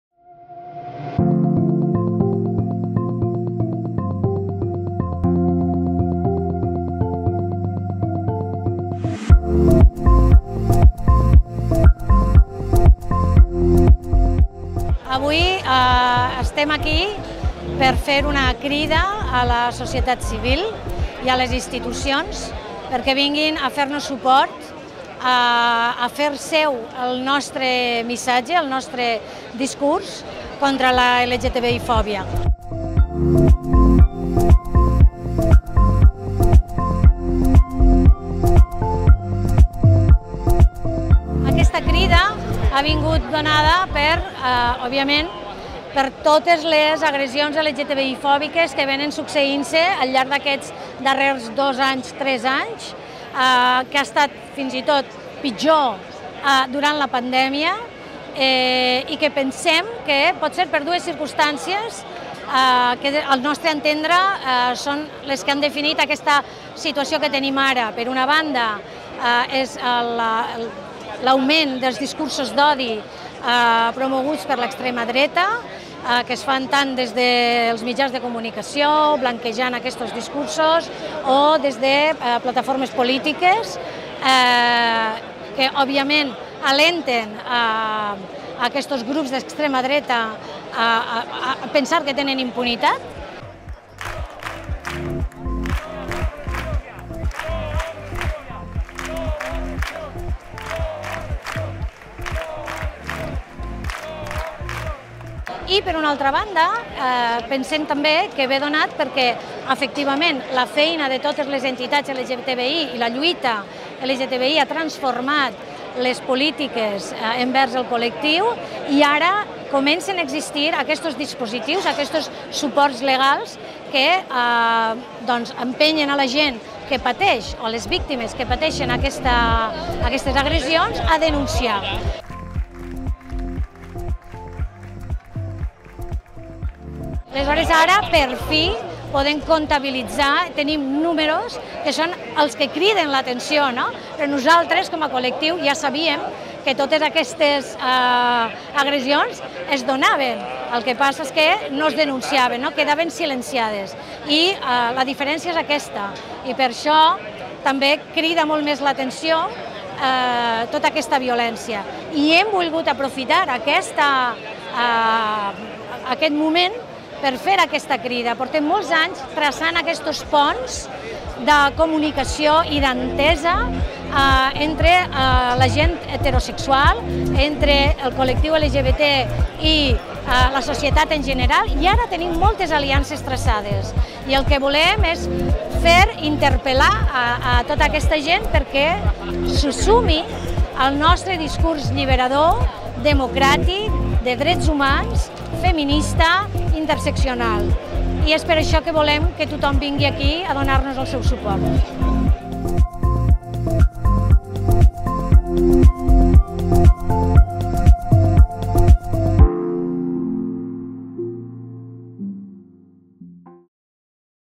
"Clam de les entitats contra la LGTBI-fòbia"
Subtitulat per a persones sordes | Segueix els nostres programes a idemtv.com

Les associacions de la Plataforma LGTBIcat han convocat a la ciutadania a manifestar-se en contra dels darrers casos de violència LGTBI-foba com el de l'assassinat de Samuel a la Corunya.

Milers de persones han sortit al carrer per denunciar la successió d'agressions que el col·lectiu ha patit les darreres setmanes.

Katy Pallàs, portaveu de la Plataforma LGTBcat ens explica que el nombre de casos (136 des del gener) es deu principalment a que s'estan denunciant les violències però que aquestes sempre han existit, la diferència és que ara els agressors s'han empoderat gràcies als discursos d'odi de la extrema dreta i que alguns mitjans blanquejen.